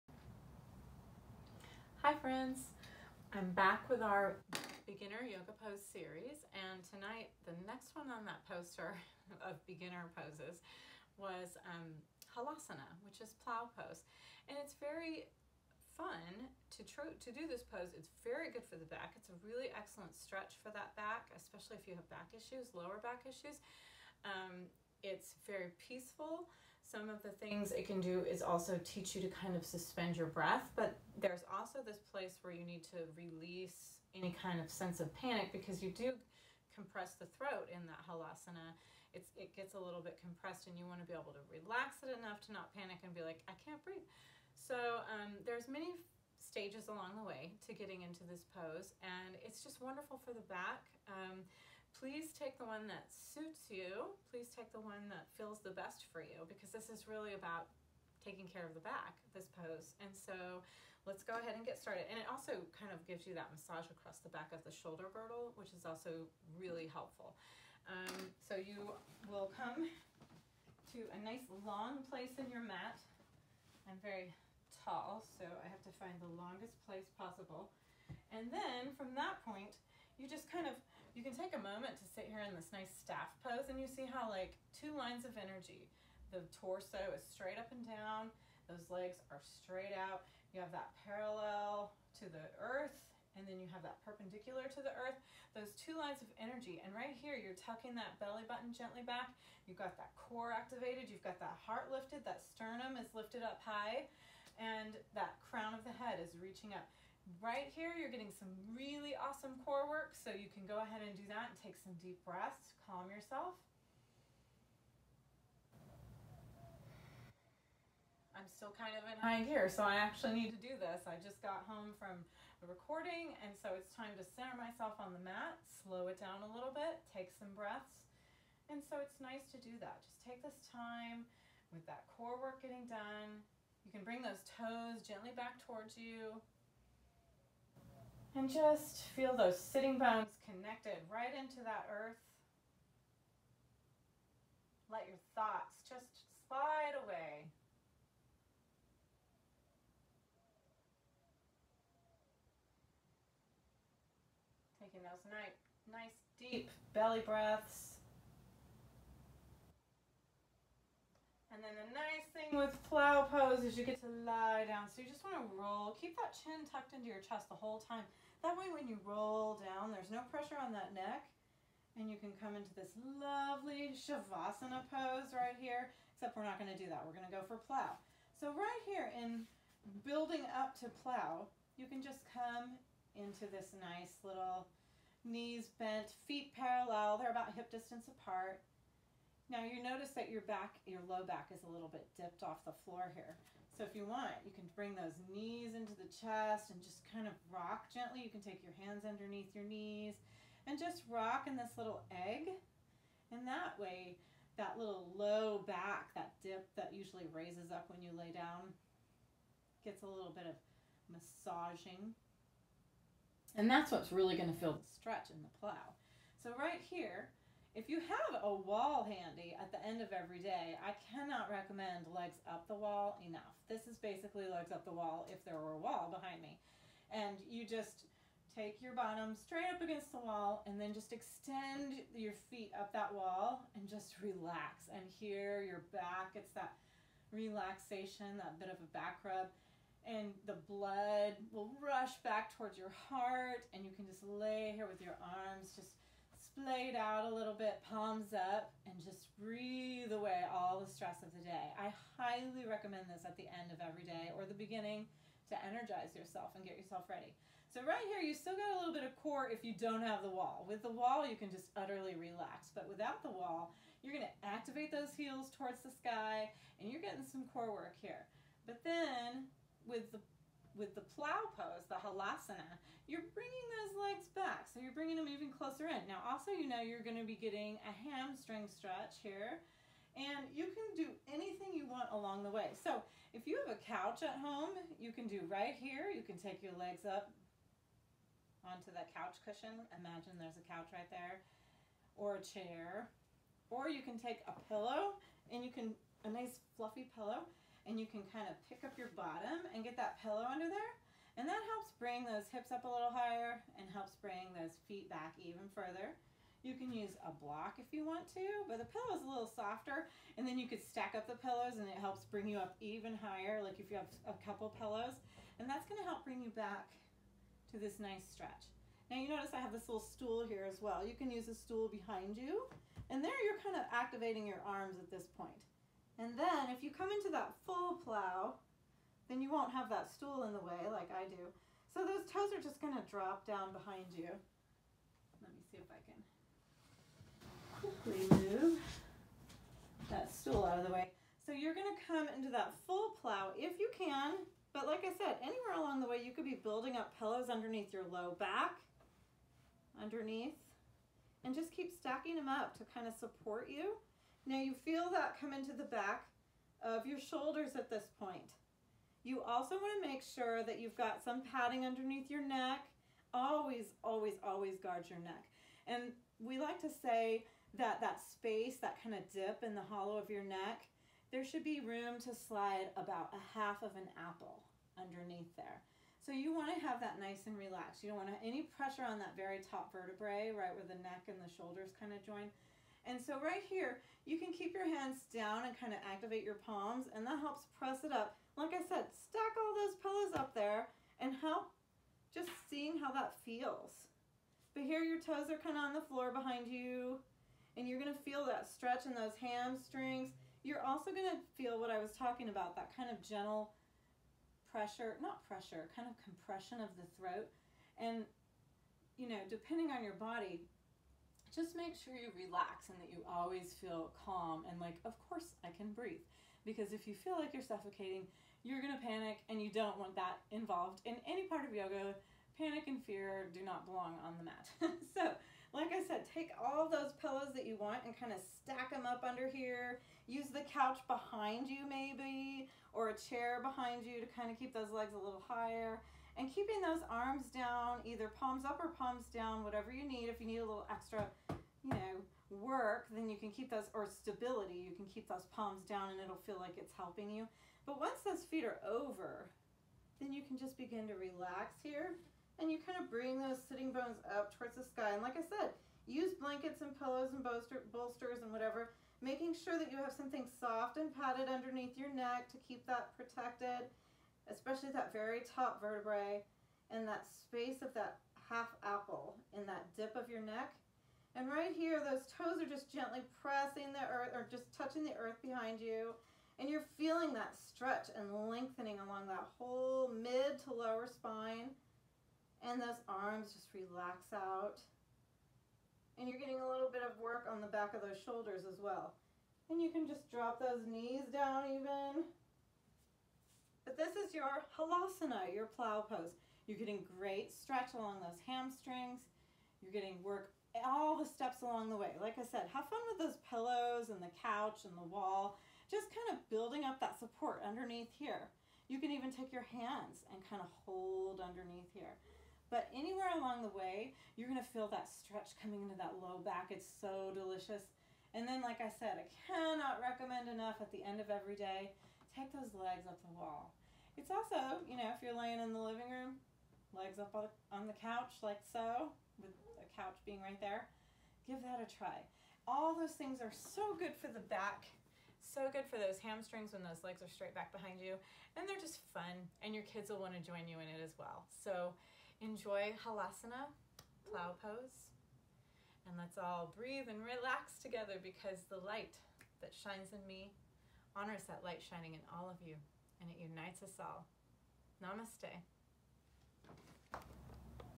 Plow pose Beginner series
The beginner pose poster series continues with plow pose!
Great for the low/mid back and hamstrings.